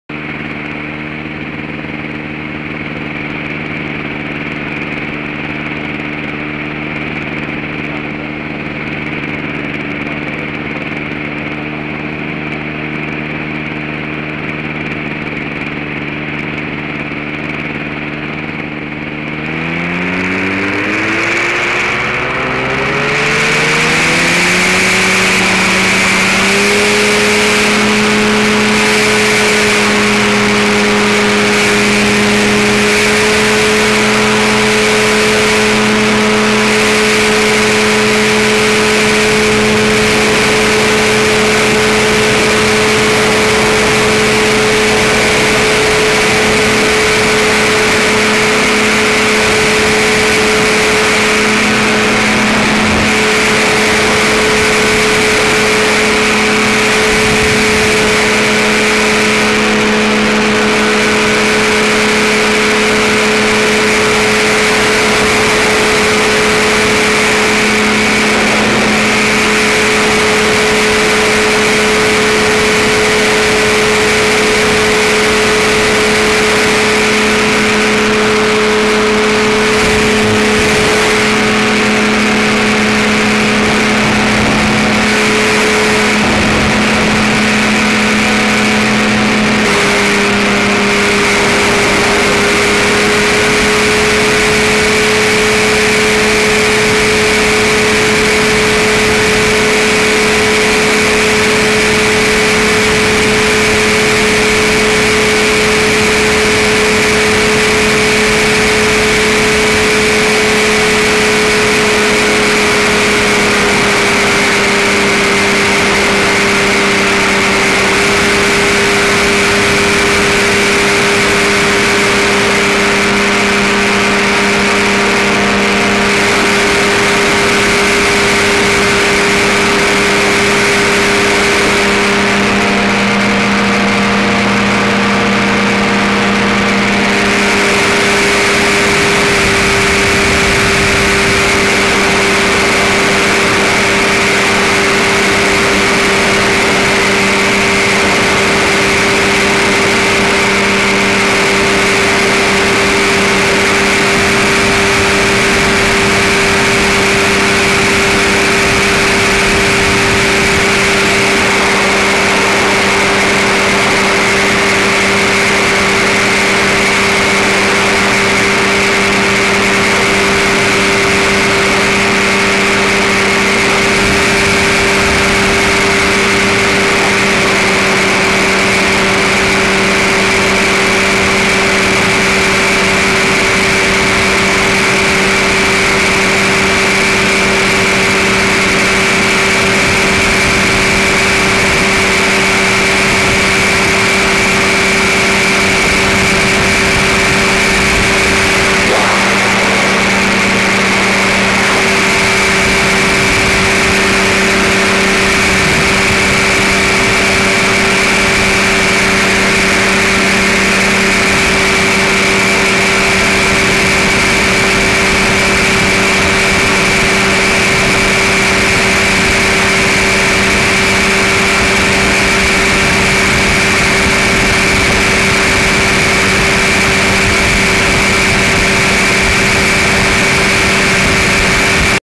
Greg MooneyTakeoff & Flight
A cut-up of a 40 minute flight showing the take-off and local views.  Greg was at the controls for about half of this flight, as evidenced by my feet in the kicked-up and relaxed position about half way through this clipo.